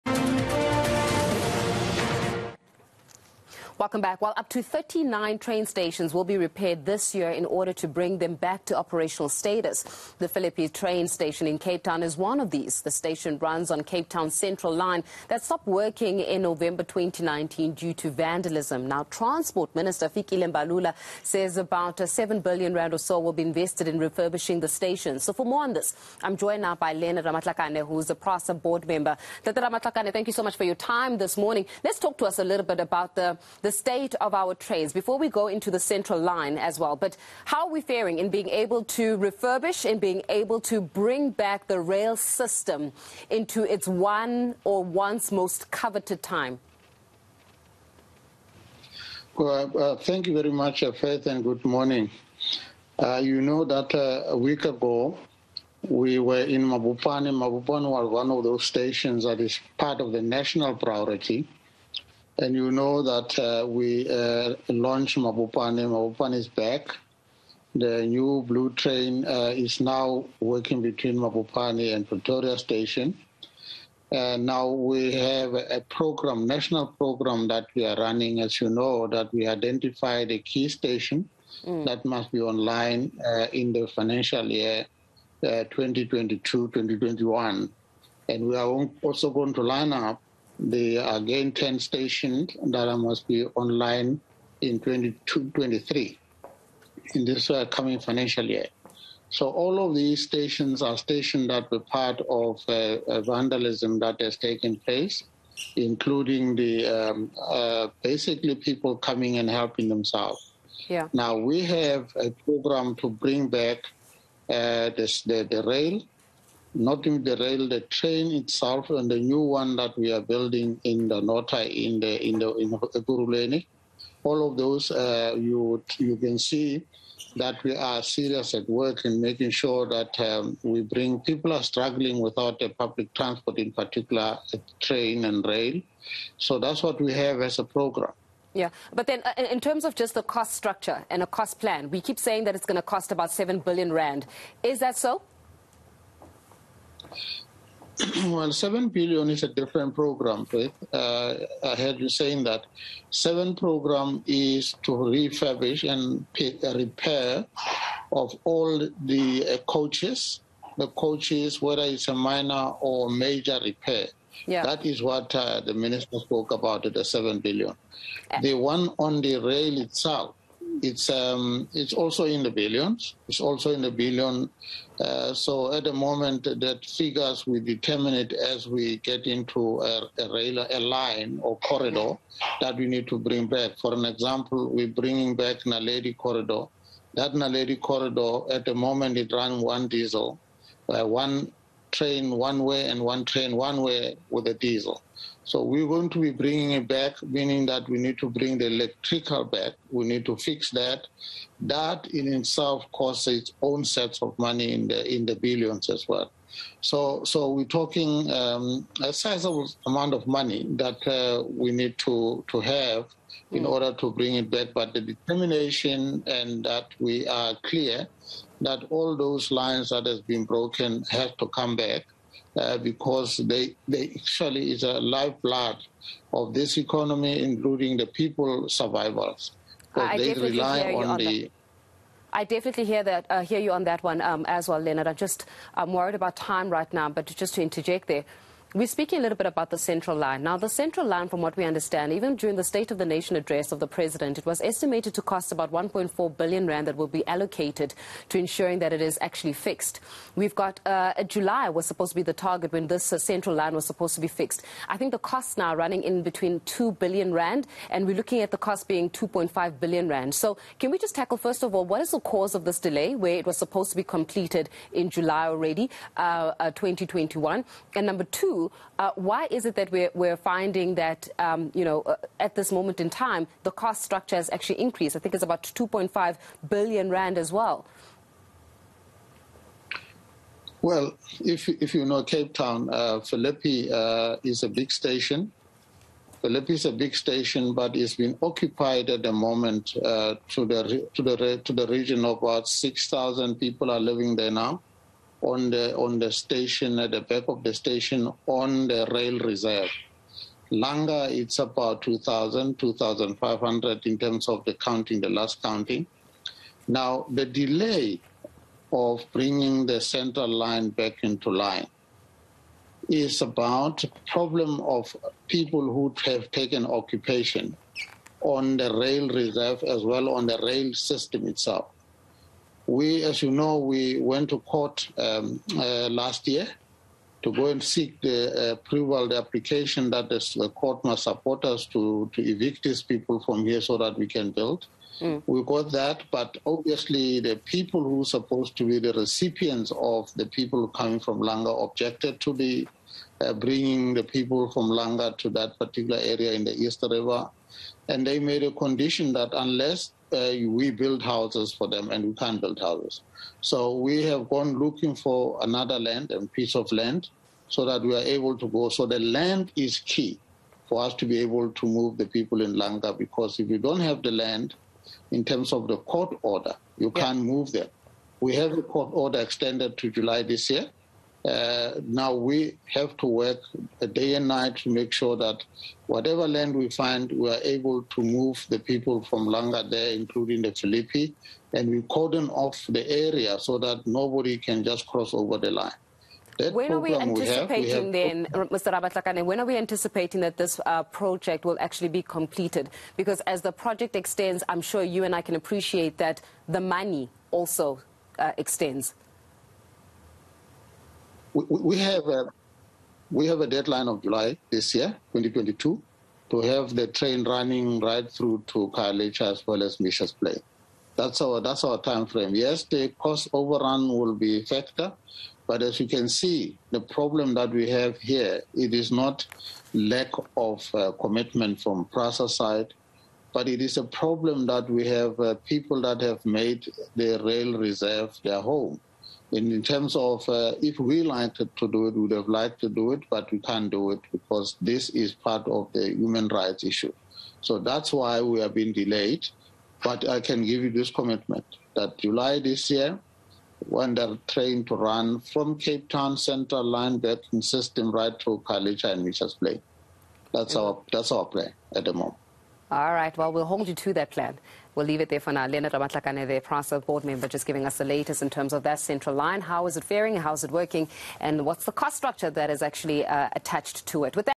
Prasa | Billions needed to repair infrastructure
Up to 39 train stations will be repaired this year in order to bring them back to operational status. The Phillipi train station in Cape Town is one of these. The station runs on Cape Town's Central Line that stopped working in November 2019 due to vandalism. Transport Minister Fikile Mbalula says R7-billion will be invested in refurbishing the stations. Courtesy